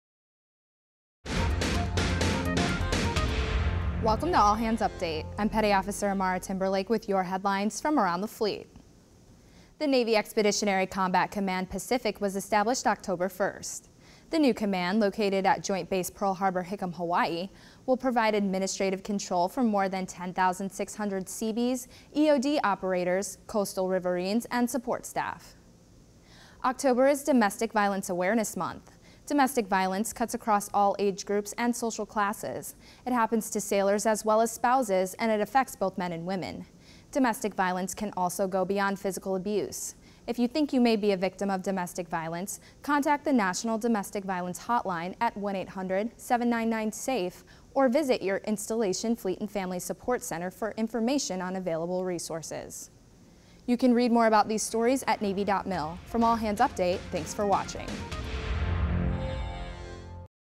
Navy Establishes New Command; Sailors Reminded of Domestic Violence Awareness Month
All Hands Update October 8th, 2012